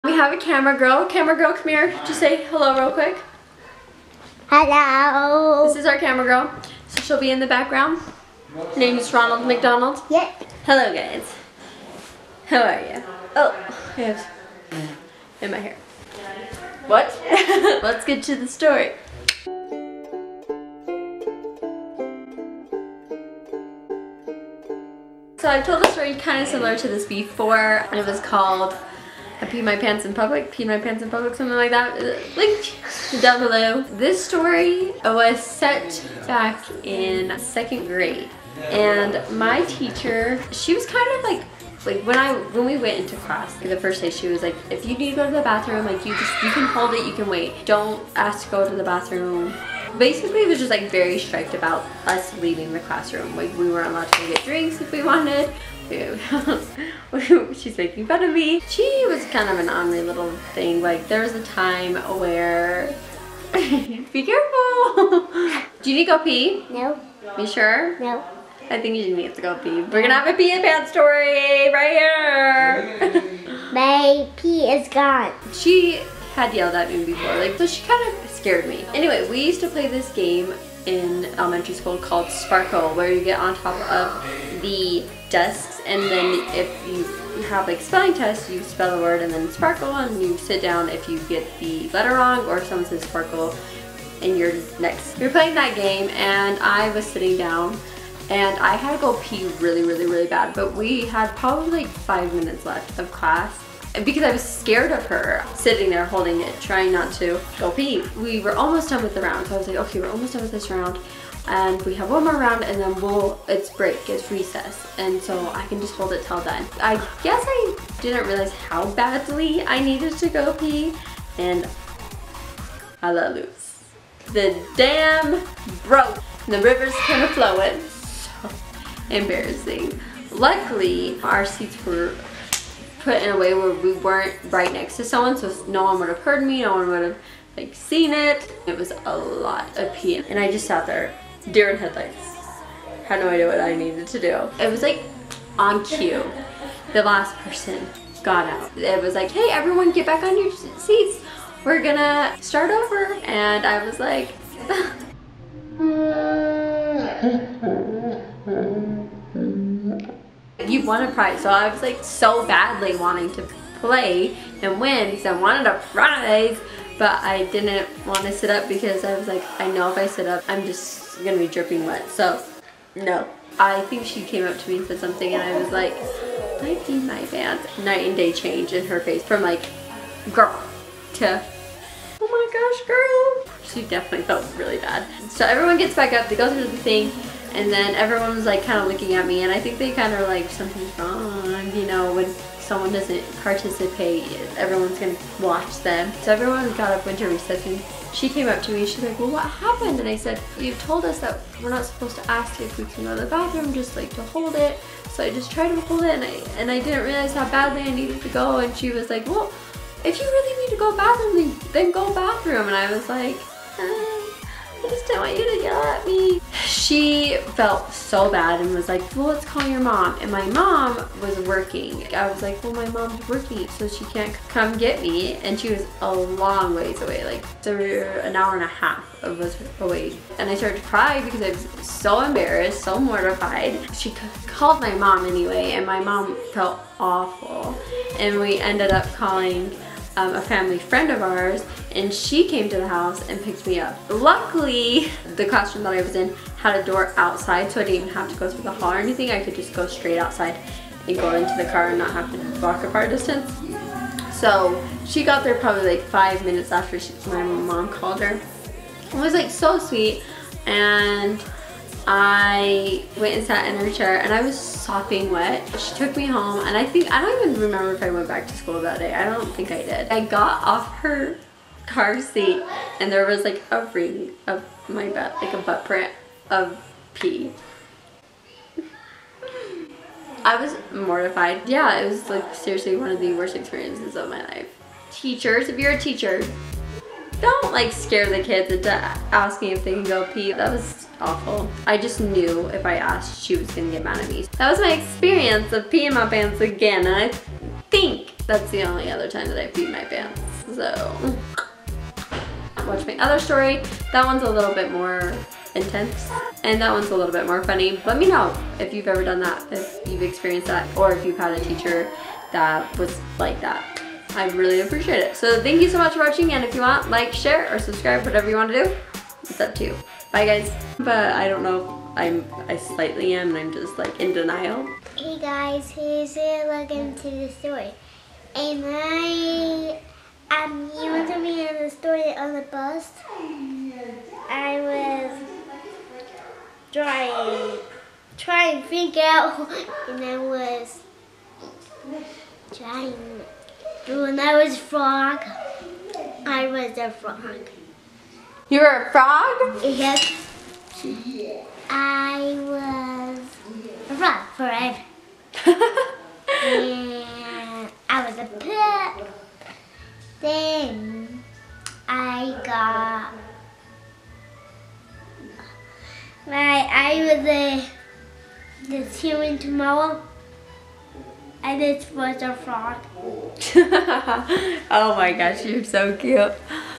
PEED MY PANTS DURING CLASS! | STORYTIME
Have you ever had that strict of a teacher?
Have you ever been too nervous to ask to go to the restroom and peed your pants or is that just me?

Hope you enjoyed and would love for you to follow us on this journey whatever you want to do its up to you!